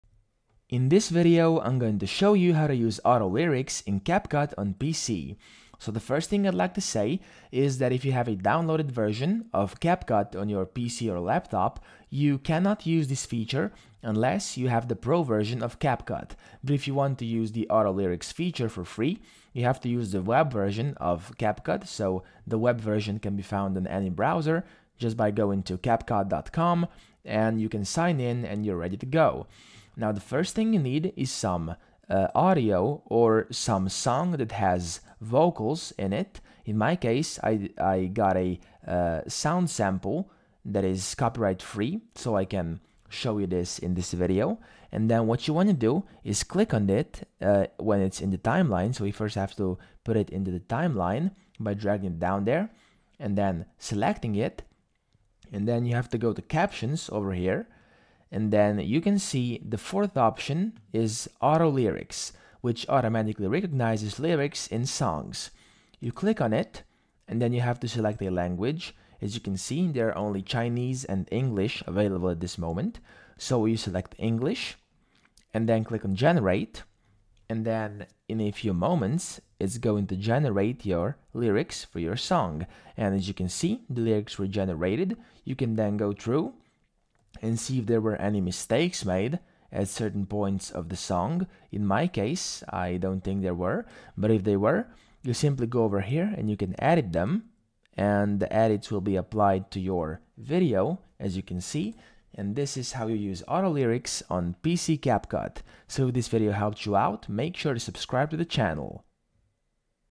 How To Use Auto Lyrics In CapCut PC (WORKING 2026)
Want to learn how to use Auto Lyrics in CapCut on your PC? In this tutorial, we'll show you step by step how to do it! Say goodbye to manual typing and hello to effortless video editing. Perfect for music videos and lyric videos!